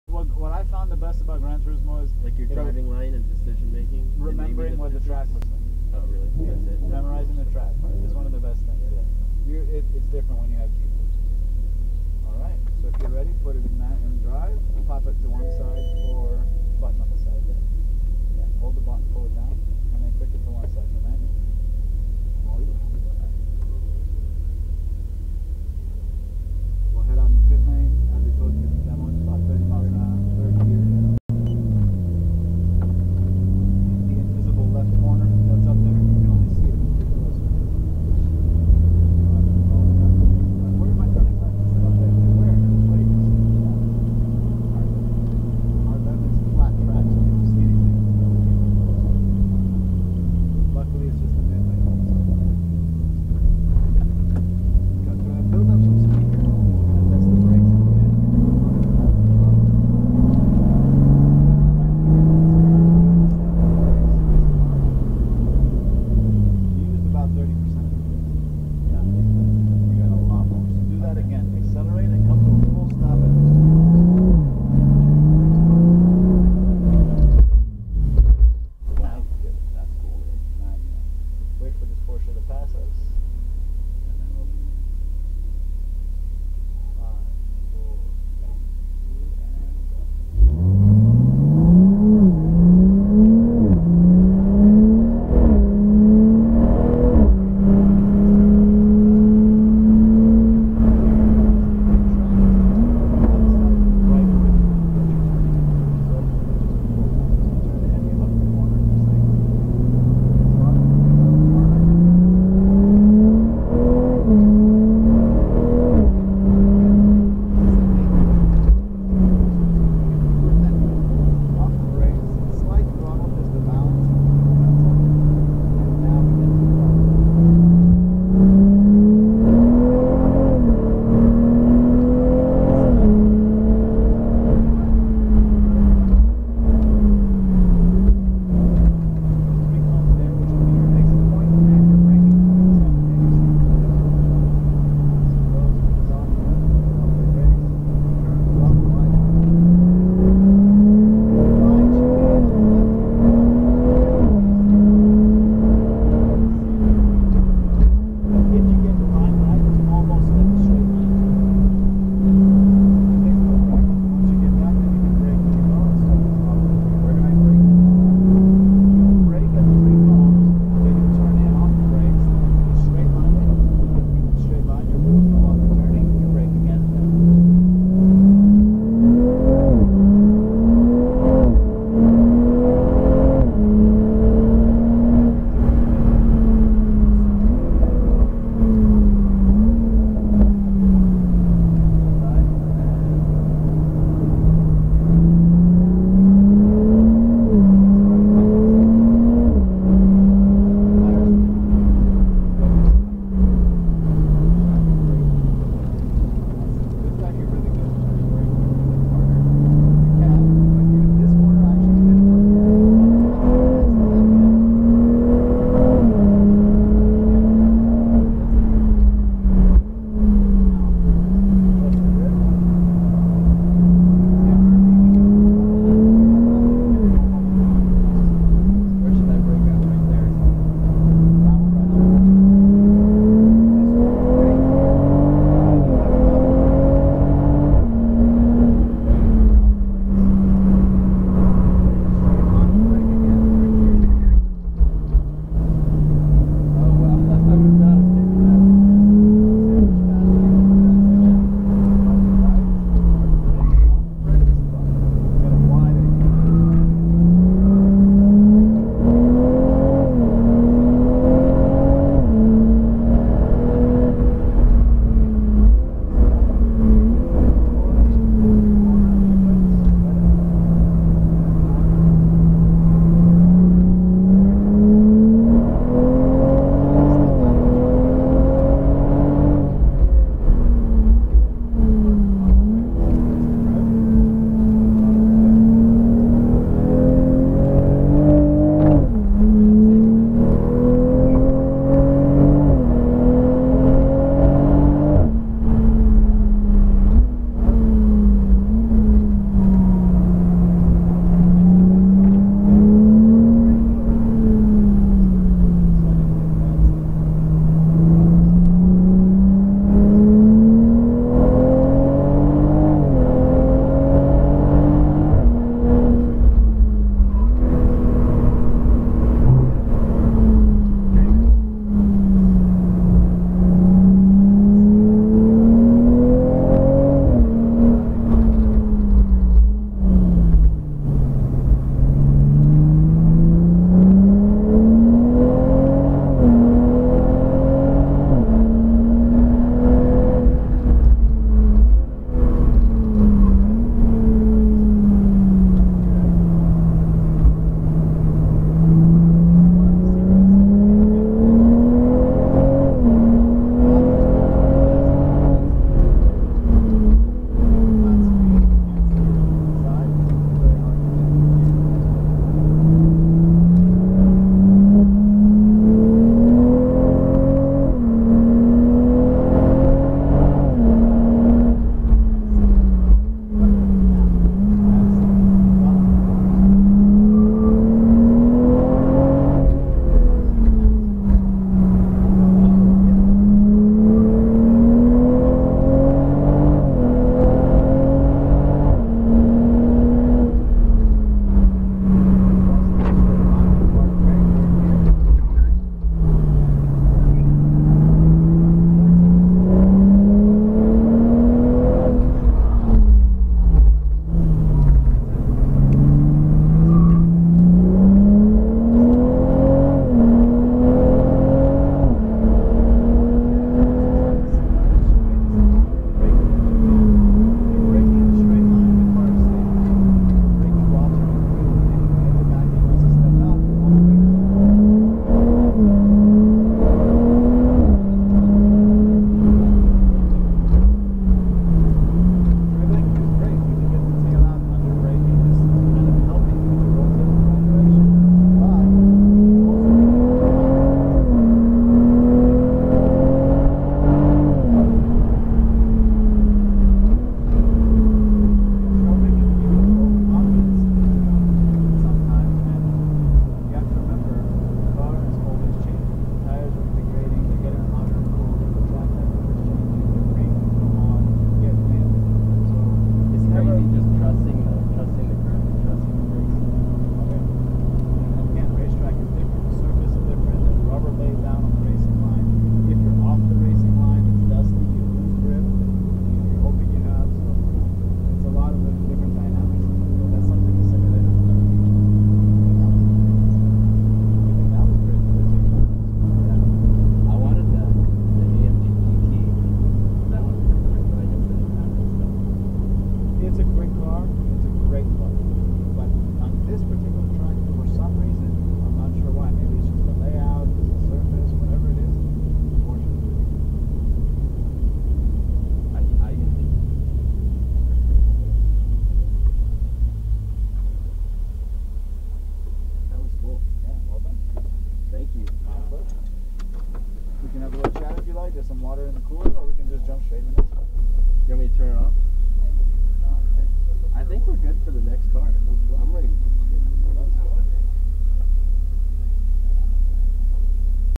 Dream Racing Las Vegas - Toyota Supra
First laps around a track ever.
Best Lap starts @ 5:20

Toyota Supra (A90)
Turbo 3.0L I-6
382hp @ 5,800rpm
0-60 mph in 3.9s